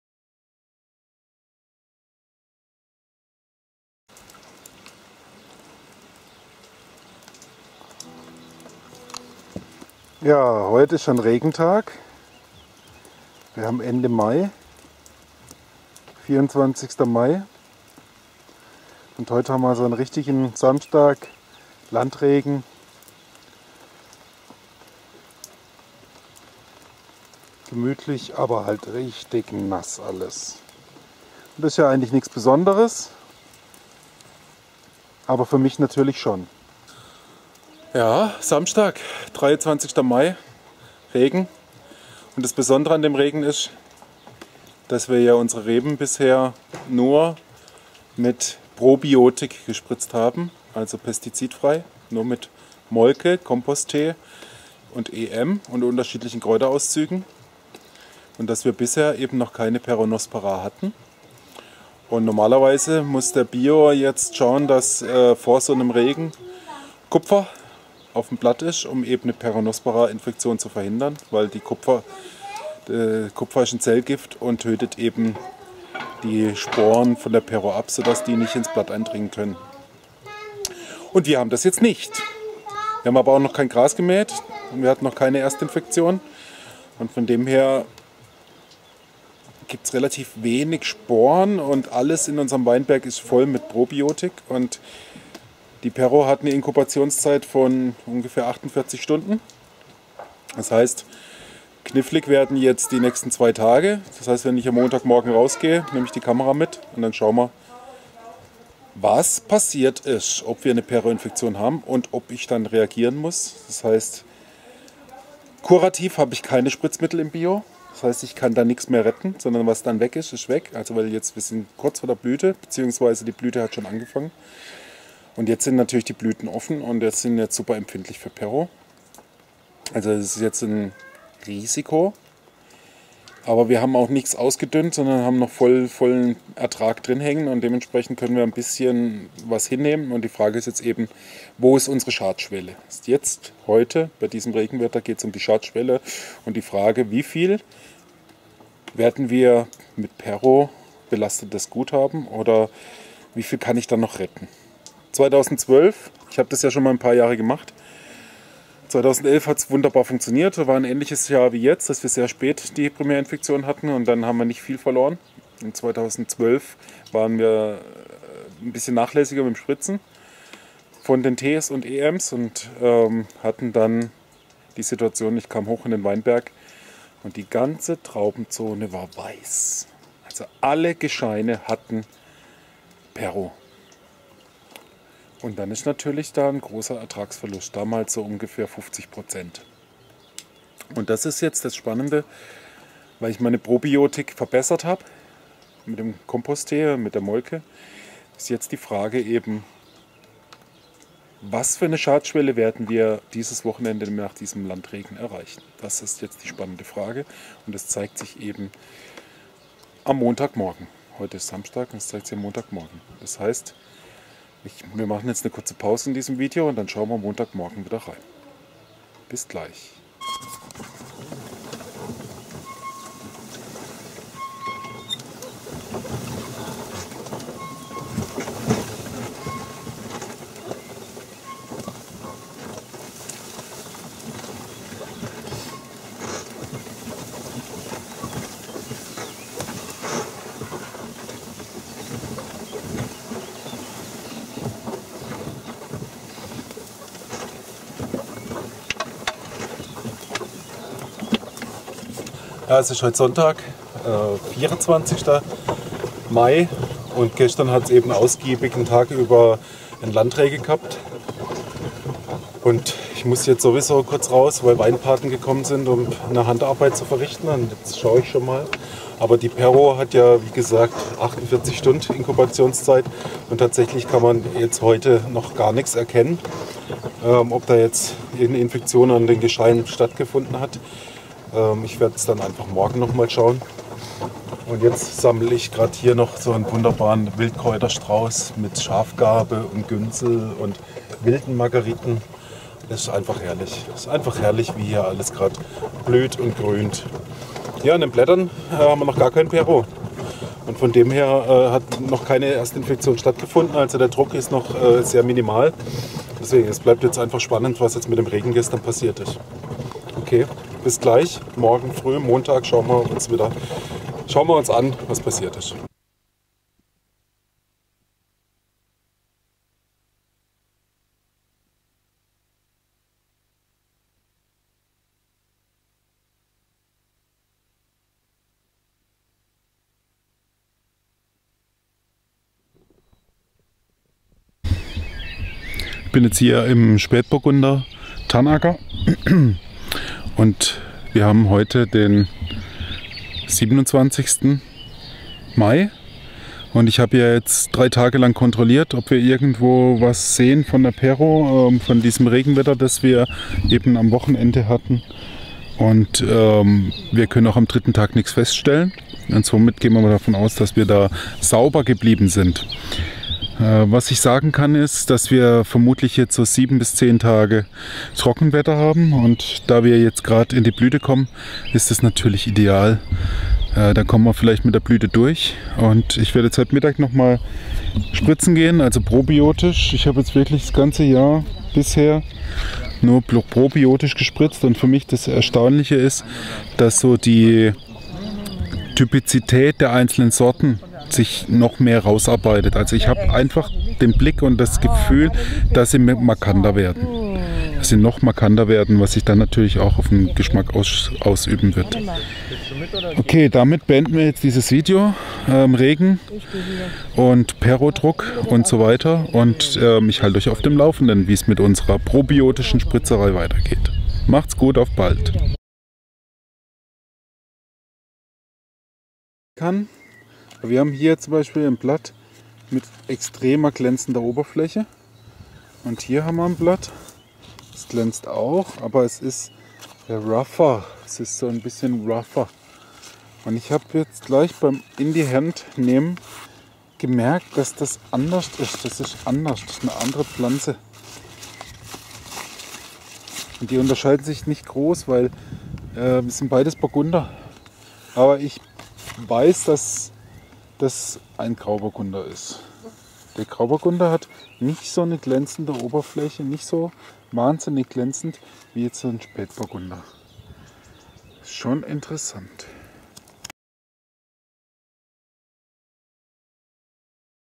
WINZERHOF LINDER - Pestizidfreie Landwirtschaft - Update 1
heute gibt es ein Update zum großen Feldversuch PROBIOTIK vs. ANTIBIOTIK in der Landwirtschaft. Es gab einen Tag lang Regen der das Peronosporainfektionsrisiko extrem erhöht hat und ich schau mir an ob eine Infektion stattgefunden hat.